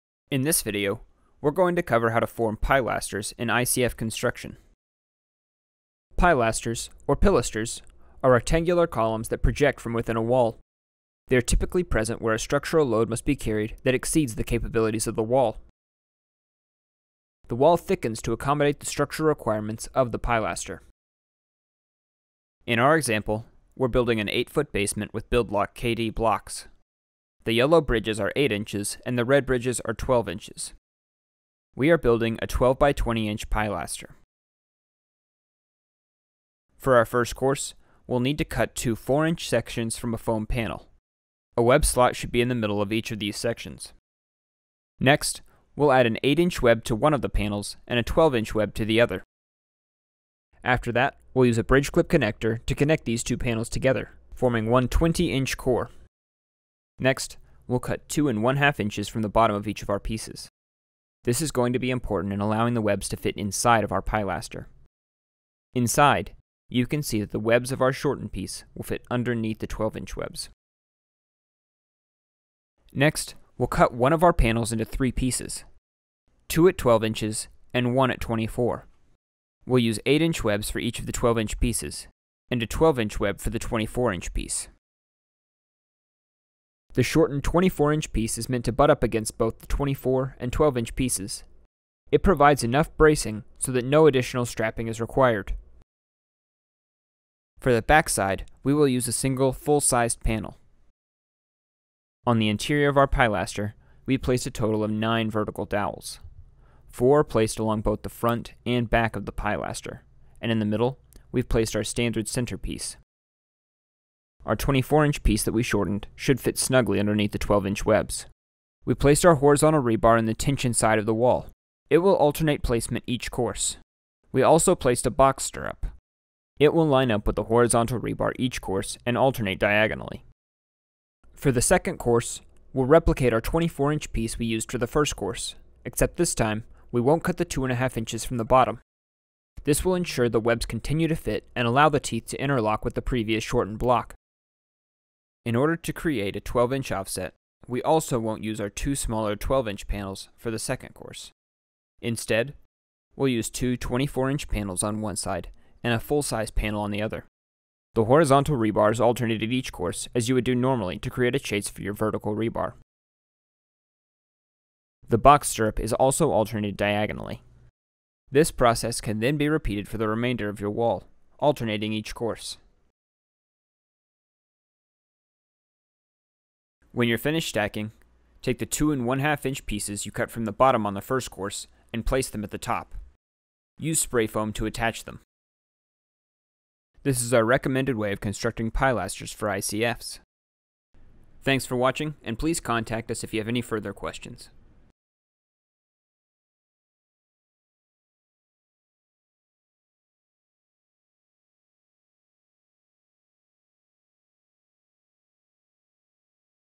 How To Construct ICF Pilasters
In this video, we show you how BuildLock KnockDown (KD) forms can be used to form integrated pilasters.